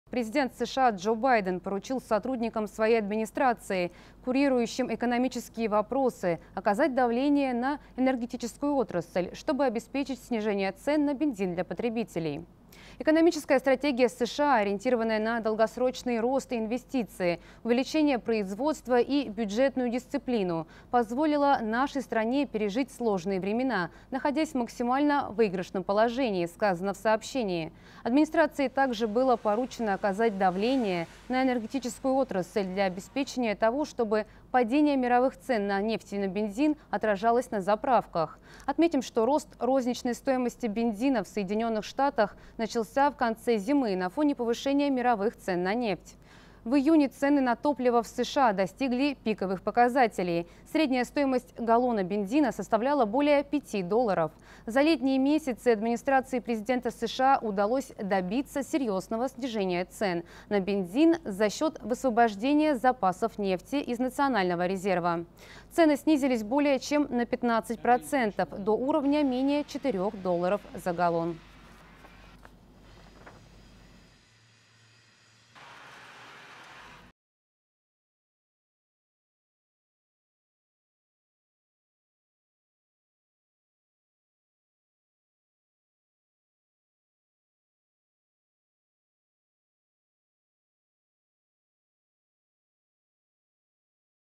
Поручение Байдена для энергетической отрасли
Президент США Джо Байден поручил сотрудникам своей администрации, курирующим экономические вопросы, "оказать давление" на энергетическую отрасль, чтобы обеспечить снижение цен на бензин для потребителей.

"Экономическая стратегия США, ориентированная на долгосрочный рост и инвестиции, увеличение производства и бюджетную дисциплину, позволила нашей стране пережить сложные времена, находясь в максимально выигрышном положении", - сказано в сообщении. Администрации также было поручено оказать давление на энергетическую отрасль для обеспечения того, чтобы падение мировых цен на нефть и на бензин отражалось на заправках. Отметим, что рост розничной стоимости бензина в Соединенных Штатах начался в конце зимы на фоне повышения мировых цен на нефть. В июне цены на топливо в США достигли пиковых показателей: средняя стоимость галлона бензина составляла более пяти долларов. За летние месяцы администрации президента США удалось добиться серьезного снижения цен на бензин за счет высвобождения запасов нефти из национального резерва. Цены снизились более чем на 15% до уровня менее четырех долларов за галлон.